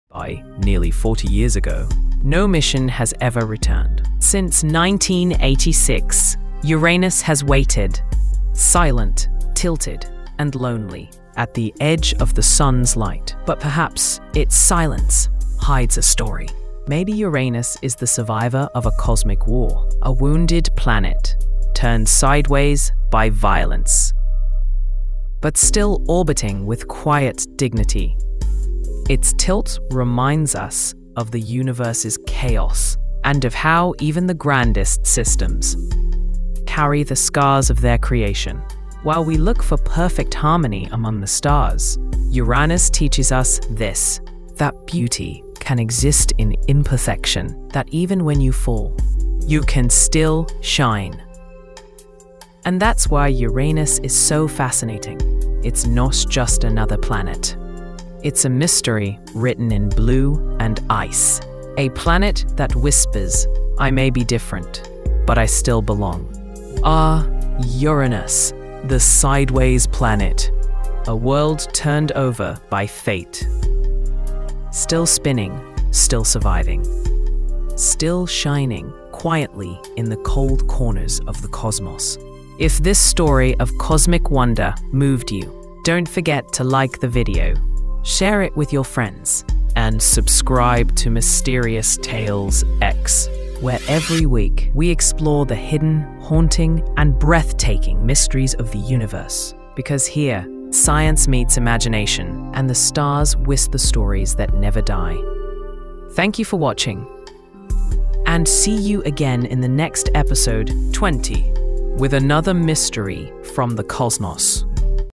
🌫️ “Uranus – The Sideways Planet” 🌫️
Among all the planets in our solar system, Uranus is the strangest.
While every other planet spins upright, Uranus rolls on its side — like a ball knocked over by a cosmic force.

Scientists believe that billions of years ago, a massive collision with an Earth-sized object tilted Uranus by 98 degrees.
This bizarre tilt causes extreme seasons — each pole faces 42 years of continuous sunlight, followed by 42 years of total darkness.

Its blue-green color comes from methane gas in its atmosphere, giving it a mysterious and icy glow.
But deep inside, Uranus hides a frozen ocean of water and ammonia — a world colder than any other planet in our system.

Even today, Uranus remains one of the least explored planets.
Silent, tilted, and strange — like a secret the universe isn’t ready to reveal yet.